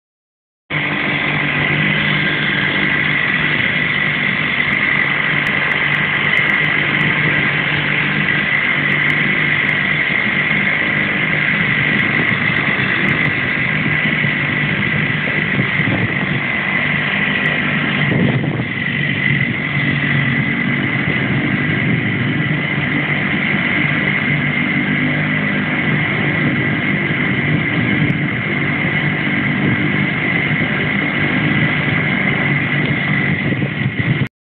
Yamaha 2hp stroke, it LIVES again!
After waking up yesterday morning to be greeted with a propellor  and a dinghy bottom, thanks to the remnant winds from Hurricaine Katia flipping Mini V in the night, the little Yamaha 2hp needed a quick dose of looking at. 45 minutes of flushing, spraying with WD40 and pulling and pulling and pulling... it sprang back into life. So this mornings commute went smoothly, with the usual sound of a swarm of hornets following me to shore. What a great little engine.